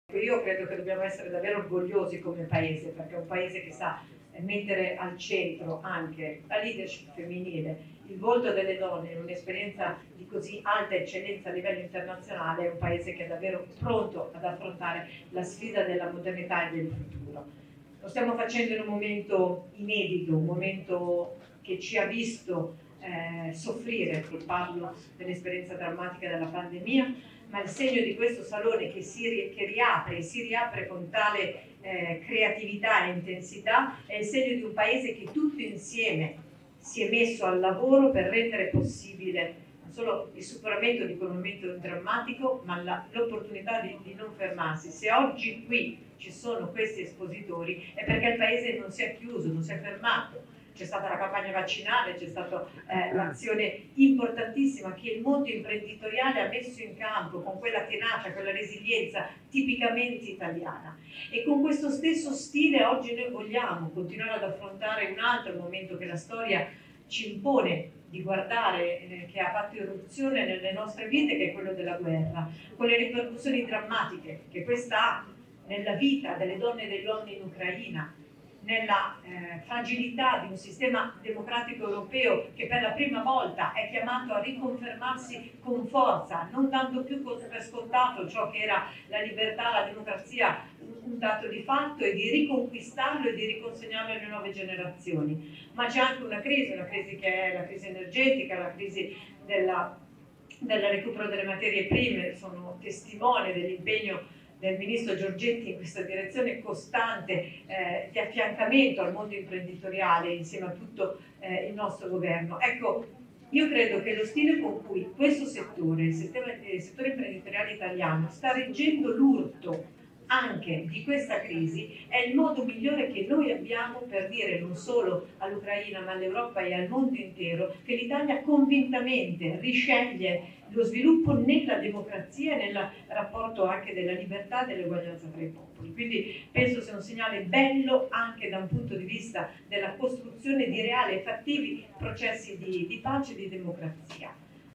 Ucraina, Bonetti: "Alla Russia diciamo che l'Italia sceglie democrazia e libertà"
L'intervento della ministra all'inaugurazione del Salone del Mobile di Milano

POLITICA (Milano). "C'è una crisi energetica e di recupero delle materie prime: lo stile con cui il settore imprenditoriale italiano sta reggendo all'urto di questa crisi è il modo migliore che abbiamo per dire non solo alla Russia ma all'Europa e al mondo intero che l'Italia convintamente sceglie lo sviluppo nella democrazia e in un rapporto di libertà e uguaglianza tra i popoli". Lo ha detto la ministra della Famiglia e delle Pari opportunità, Elena Bonetti, durante la cerimonia inaugurale della 60esima edizione del Salone del Mobile di Milano. "Penso sia un segnale bello anche nella costruzione di processi reali e fattivi di pace", ha aggiunto. (Alessandro Boldrini/alanews)

Io credo che dobbiamo essere orgogliosi come fai un paese e mettere al centro anche la leadership femminile il volto delle donne un'esperienza di così alta eccellenza a livello internazionale paese che davvero pronto ad affrontare la sfida della modernità in un momento i medici Un momento che ci ha visto dalla parte mia e di questo salone che si riapre si riapre con creatività e intensità è serio il tuo paese che tu sia al lavoro per rendere possibile il superamento di commento drammatico Ma se oggi qui ci sono questi espositori importantissima che il mondo imprenditoriale adesso in campo questo stesso stile oggi noi vogliamo continuare ad affrontare un altro momento che la storia ci impone di guardare che ha fatto irruzione nelle nostre vite che quello che sta nella vita delle donne in Ucraina nella fragilità di un sistema Democratico europeo e per la prima volta con forza non tanto più ascoltato ciò che era la libertà la democrazia stato di fatto e di riconquistando di riconsegnare le nuove generazioni del recupero delle materie prime sono l'impegno imprenditoriale insieme a tutto il nostro governo Ecco io credo che lo stile con cui questo settore sta leggendo l'urto anche questa crisi è il modo migliore che noi abbiamo per dire non solo all'Europa e al mondo intero e glie lo sviluppo nel rapporto anche della Libertà te lo vogliono sapere che bello anche da un punto di vista